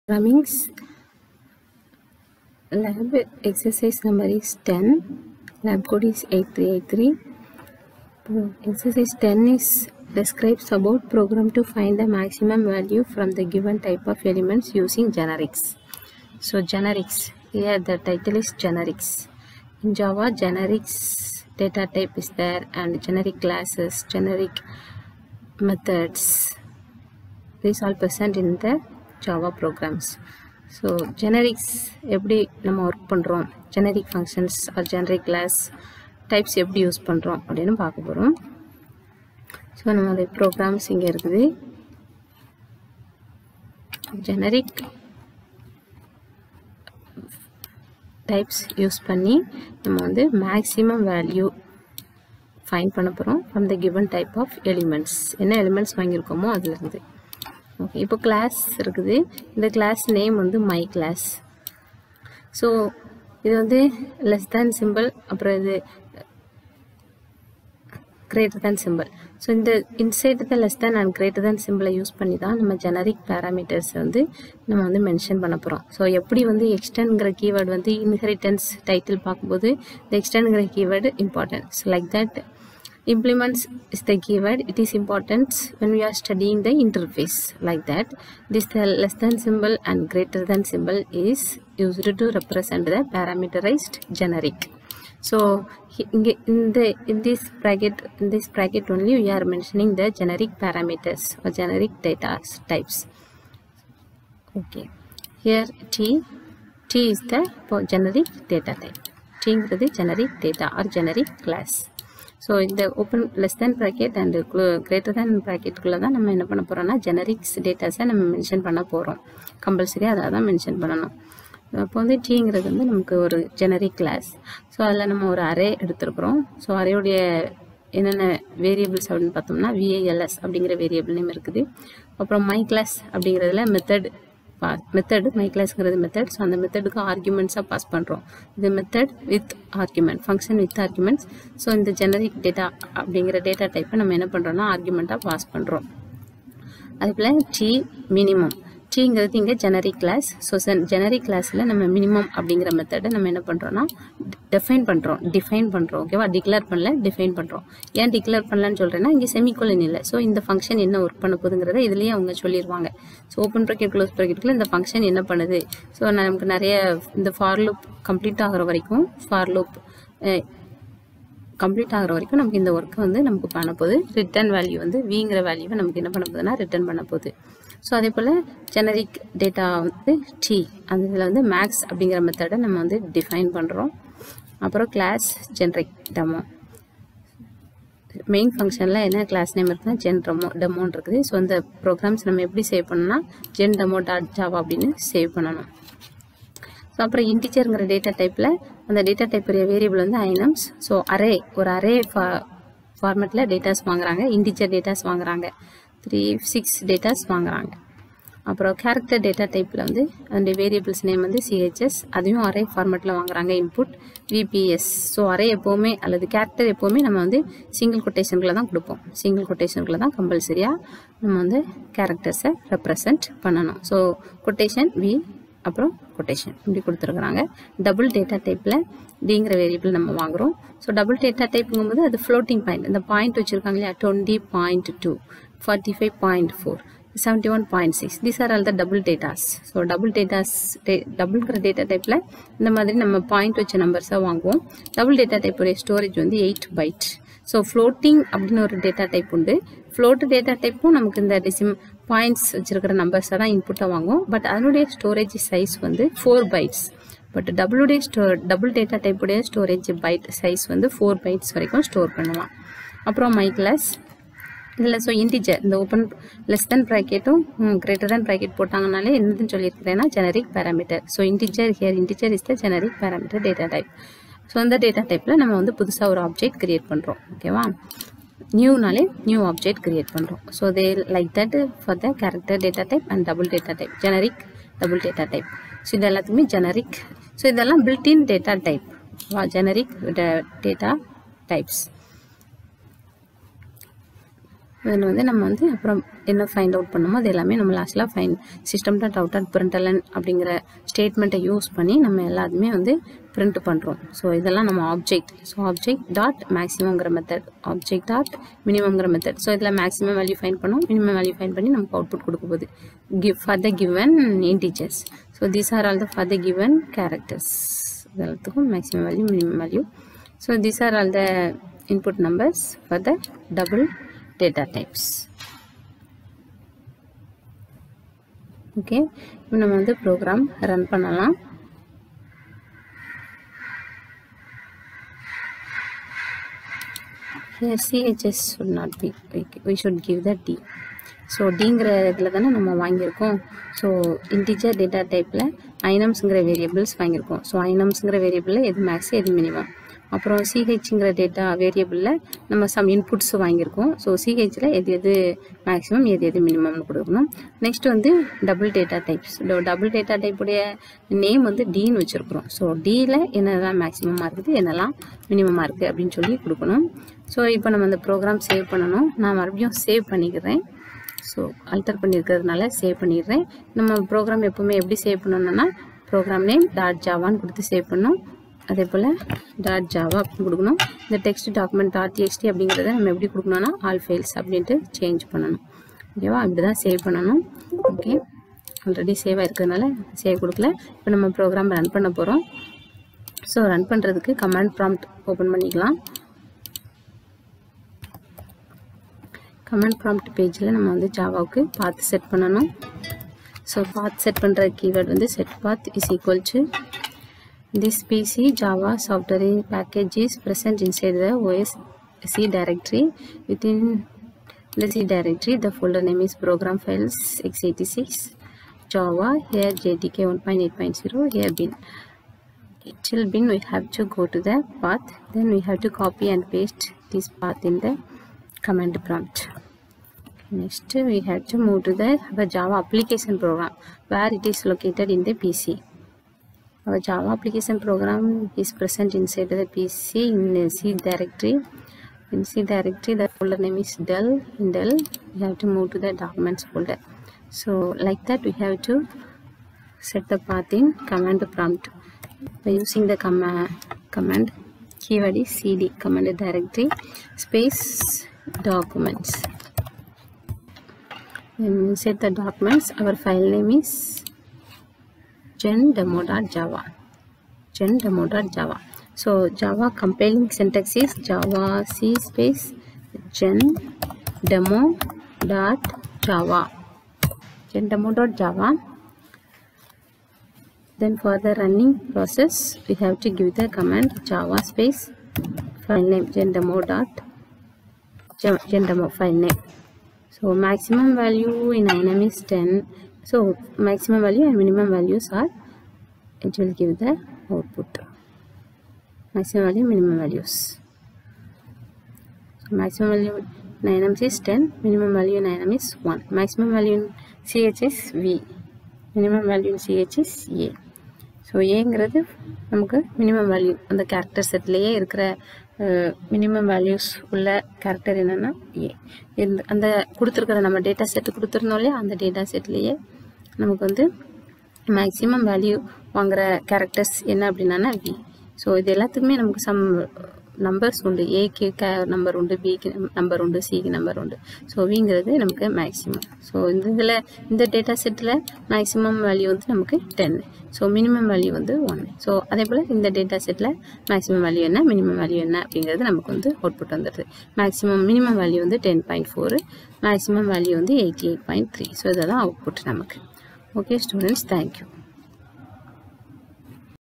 GRACE COE – CSE – CS 8383 – OO PROGRAMMING LAB – Ex No :10 – Find Maximum Value Using JAVA Generics
Ex No : 10 – Find Maximum Value Using JAVA Generics

1)Open command prompt.Set path =”Copy and paste where java -jdk-bin located in pc”
2)Using CD (command directory)..cd documents ,move directory to where our java application program ie ebill.java located.
3)Compile using the syntax javac filename.java
4)Run using the syntax java filename
5)Filename should be same as class name.